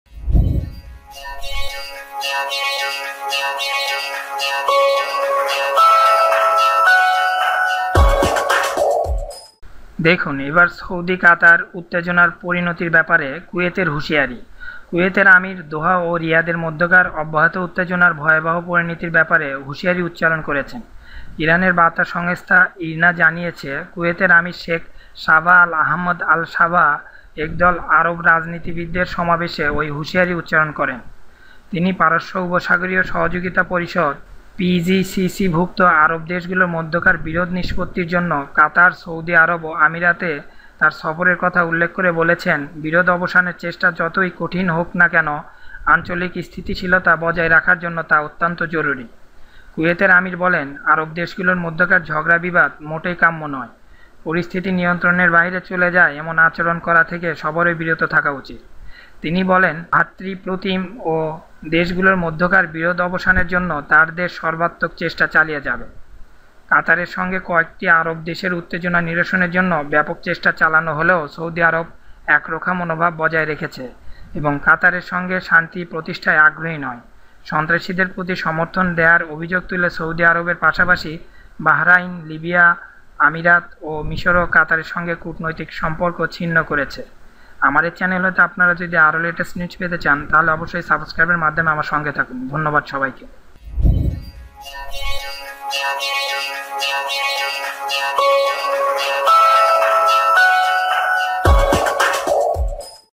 দেখুন এবার সৌদি কাতার উত্তেজনার পরিণতির ব্যাপারে কুয়েতের হুঁশিয়ারি
কুয়েতের আমির দোহা ও রিয়াদের মধ্যেকার অব্যাহত উত্তেজনার ভয়াবহ পরিণতির ব্যাপারে হুঁশিয়ারী উচ্চারণ করেছেন।
ইরানের বার্তা সংস্থা ইরনা জানিয়েছে, কুয়েতের আমির শেখ সাবাহ আল আহমাদ আল সাবাহ একদল আরব রাজনীতিবিদের সমাবেশে ওই হুঁশিয়ারি উচ্চারণ করেন। তিনি পারস্য উপসাগরীয় সহযোগিতা পরিষদ বা পিজিসিসিভুক্ত আরব দেশগুলোর মধ্যকার বিরোধ নিষ্পত্তির জন্য কাতার, সৌদি আরব ও আমিরাতে তার সফরের কথা উল্লেখ করে বলেছেন, বিরোধ অবসানের চেষ্টা যত কঠিনই হোক না কেন আঞ্চলিক স্থিতিশীলতা বজায় রাখার  জন্য তা অত্যন্ত জরুরি।

কুয়েতের আমির বলেন, আরব দেশগুলোর মধ্যকার ঝগড়া-বিবাদ মোটেই কাম্য নয় এবং পরিস্থিতি নিয়ন্ত্রণের বাইরে চলে যায় এমন আচরণ করা থেকে সবারই বিরত থাকা উচিত। তিনি বলেন, ভ্রাতৃপ্রতিম এ দেশগুলোর মধ্যকার বিরোধ অবসানের জন্য তার দেশ সর্বাত্মক চেষ্টা চালিয়ে যাবে।

কাতারের সঙ্গে কয়েকটি আরব দেশের উত্তেজনা নিরসনের জন্য ব্যাপক চেষ্টা চালানো হলেও সৌদি আরব একরোখা মনোভাব বজায় রেখেছে এবং কাতারের সঙ্গে শান্তি প্রতিষ্ঠায় আগ্রহী নয়। সন্ত্রাসীদের প্রতি সমর্থন দেয়ার অভিযোগ তুলে সৌদি আরবের পাশাপাশি বাহরাইন, লিবিয়া, আমিরাত ও মিশরও কাতারের সঙ্গে কূটনৈতিক সম্পর্ক ছিন্ন করেছে।